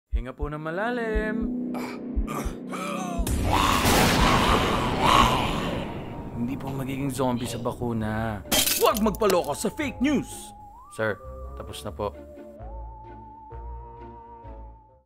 BakunaNow: Magiging zombie ka ba after your vaccination?
Takot ka ba magpa-bakuna dahil magiging zombie ka daw? 🧟‍♀️ Naku, ‘wag tayo magpaloko sa fake news!🙅‍♀️🙅‍♂️ Get yourself and your families vaccinated now. Be informed about vaccination facts and other COVID updates through legitimate sources only. Sama-sama tayo, ingat angat, bakuna lahat.